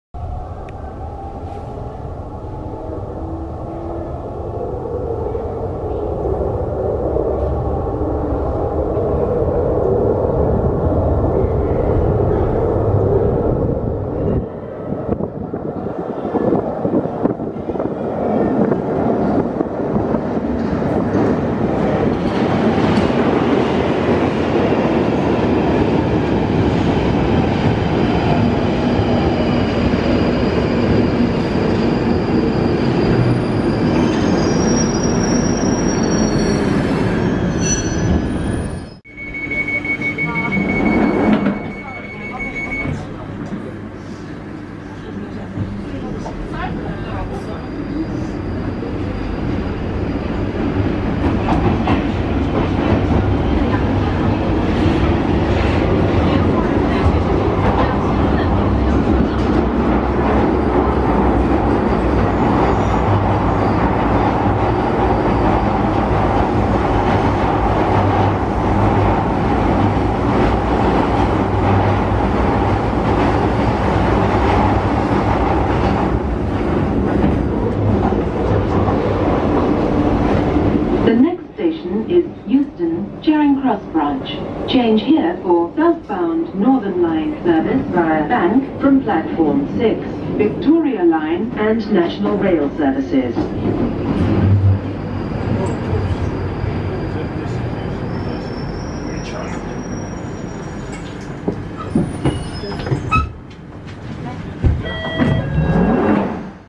London Underground Northern Line Journey from Warren Street to Euston
This video shows a journey onboard a northern line tube train heading northbound from Warren Street to Euston.